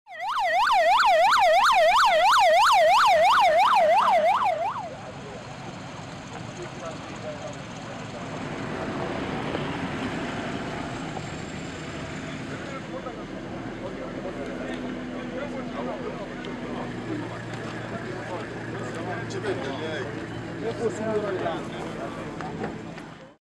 Raw: Attackers Kill 2 at Hotel Resort in Mali
Jihadis attacked a hotel resort Sunday in Mali's capital, taking hostages at a spot popular with foreigners on the weekends. More than 30 people managed to escape though at least two people were killed, authorities said. (June 18)